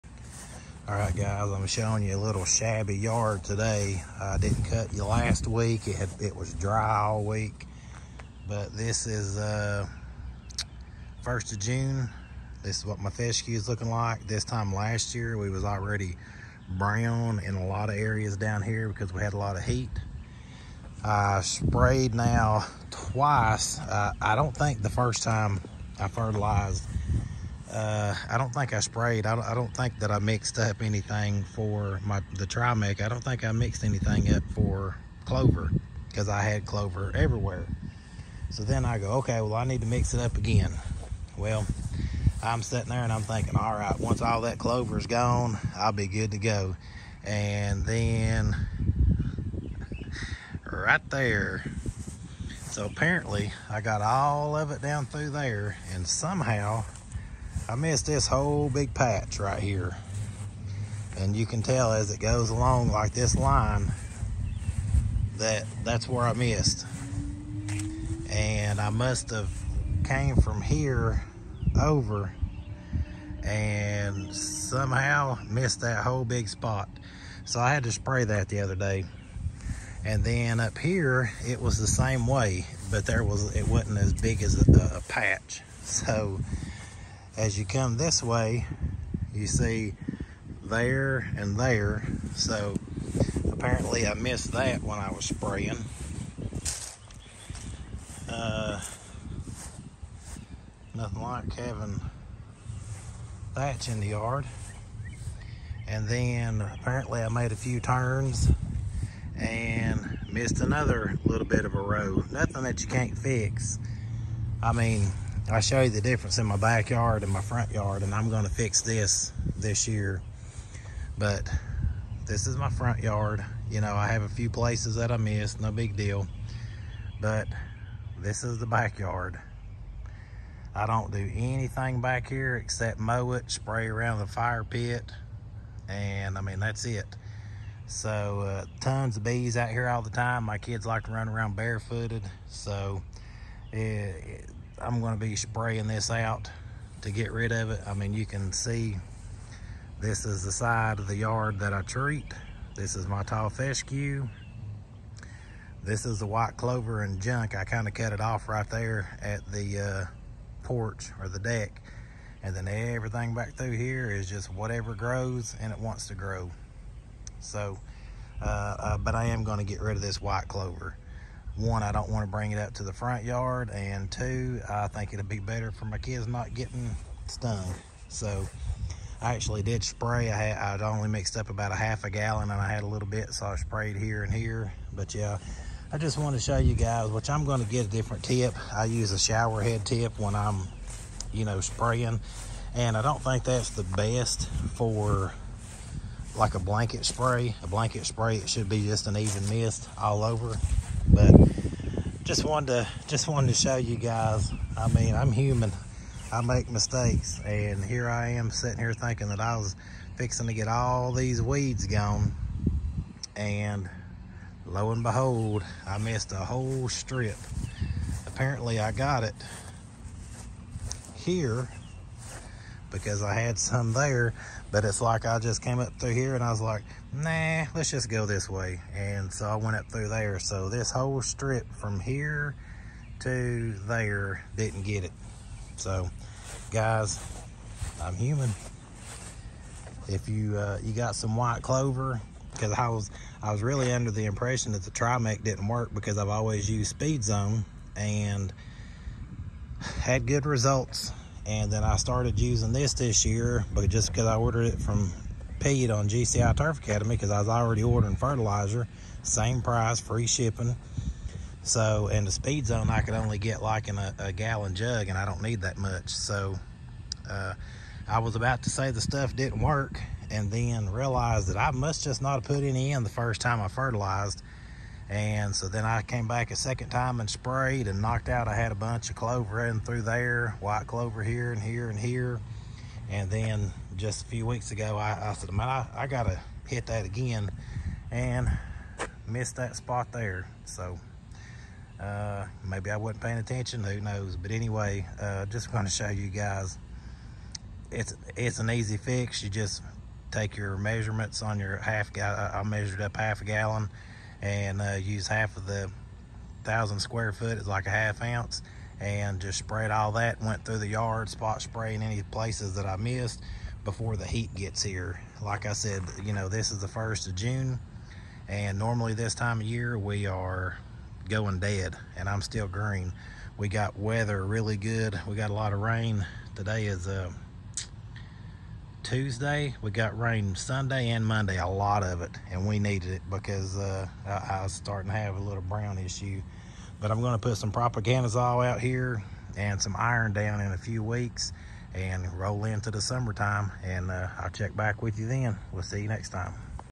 How did I miss these weeds?
In this video I show some spots in my yard that didnt get sprayed when I sprayed for weeds